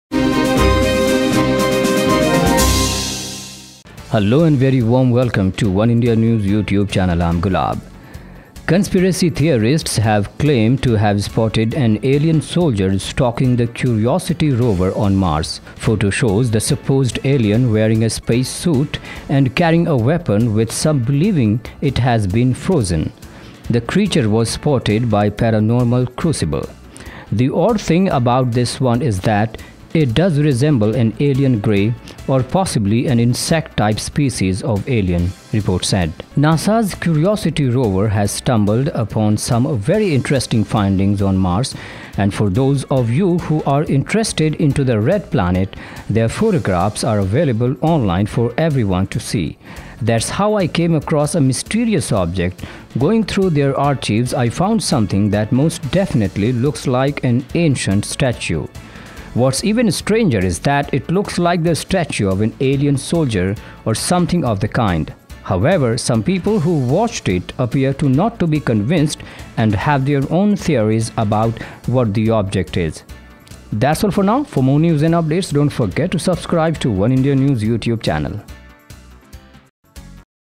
Alien Soldier spotted on Mars by NASA Curiosity Rover | Oneindia News
CONSPIRACY theorists have claimed to have spotted an Alien soldier stalking the Curiosity Rover on Mars. Photo shows the supposed alien wearing a space suit and carrying a weapon with some believing it has been “frozen”. The creature was spotted by Paranormal Crucible. As per reports the odd thing about this one is that it does resemble an alien grey or possibly an insect type species of alien.